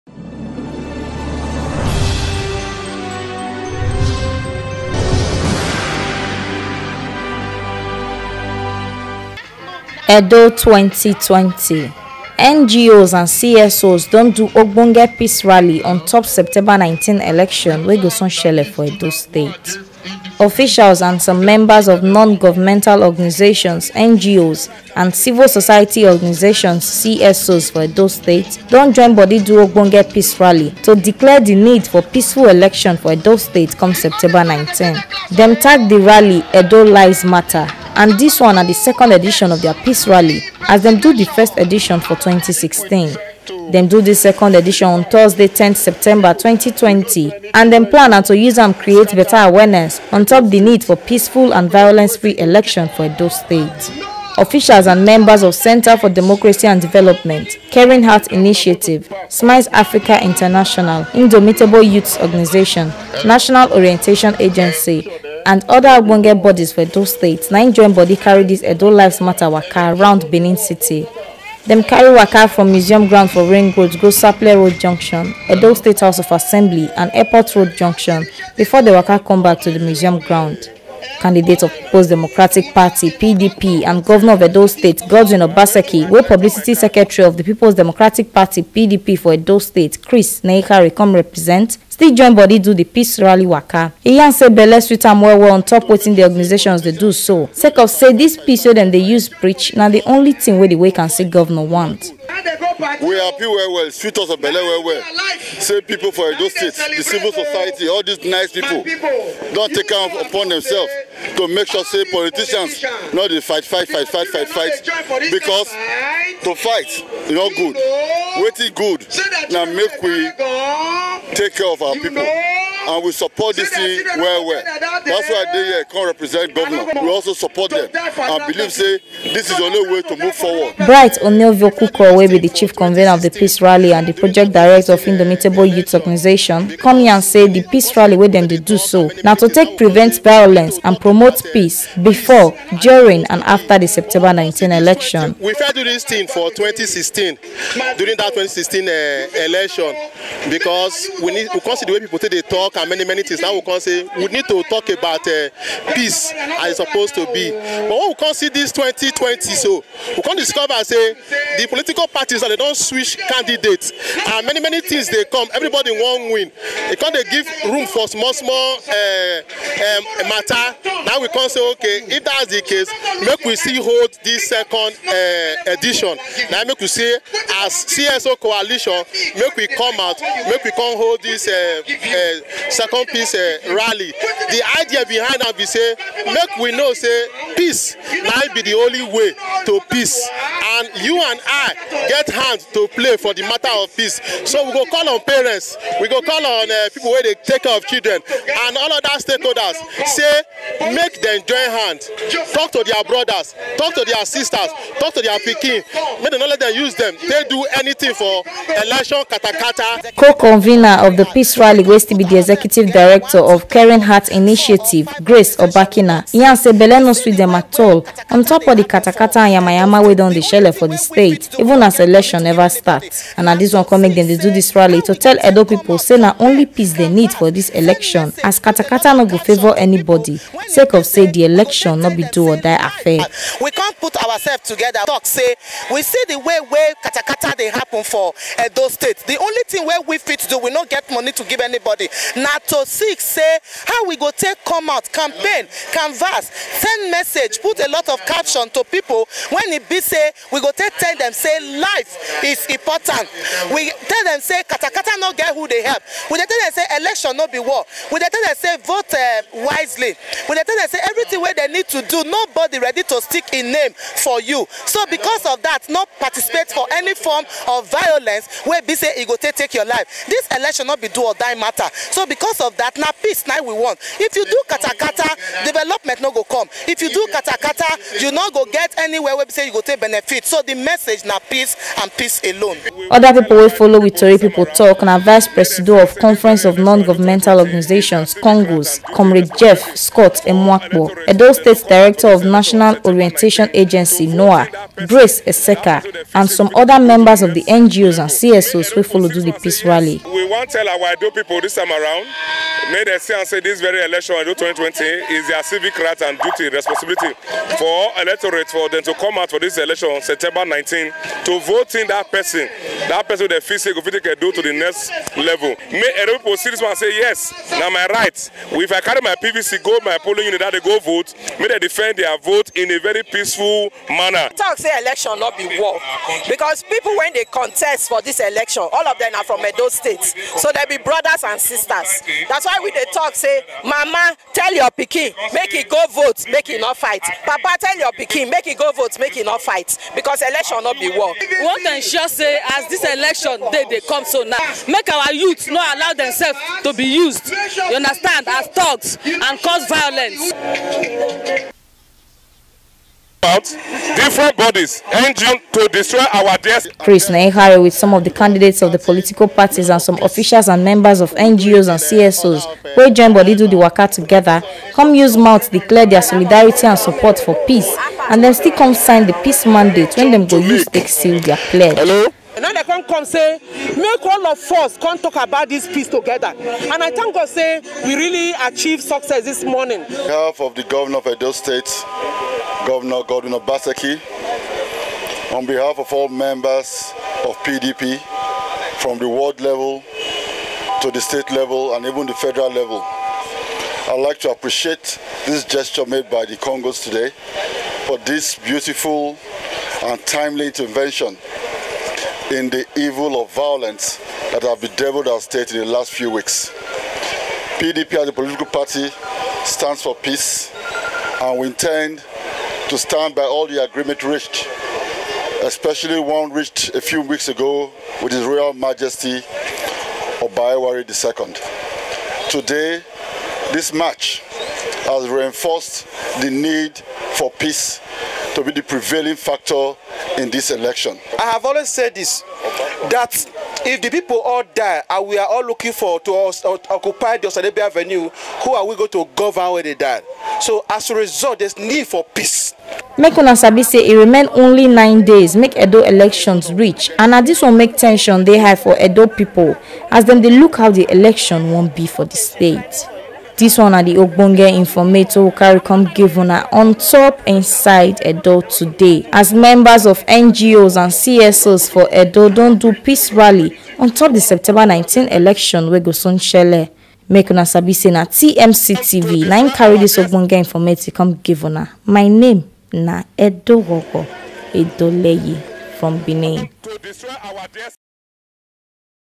EDO 2020: NGOs And CSOs Don Do Ogbonge Rally To Promote Peace Onto Edo Election Wey Go Soon Shele
and log onto to dey get more ogbonge updates